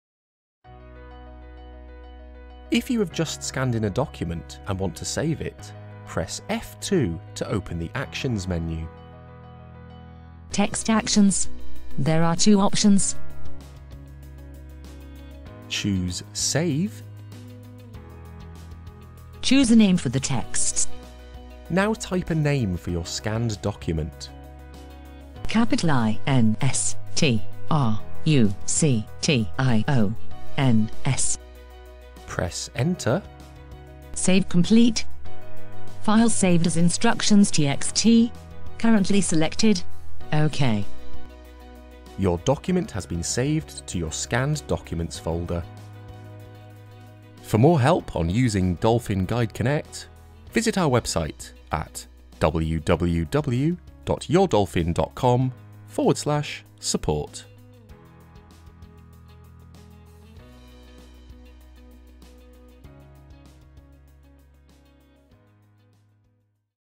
Learn GuideConnect: Scanner and Camera - Save Scanned Document
Scan and save your letters, bank statements, or medical records with Guideconnect, and read with high contrast text.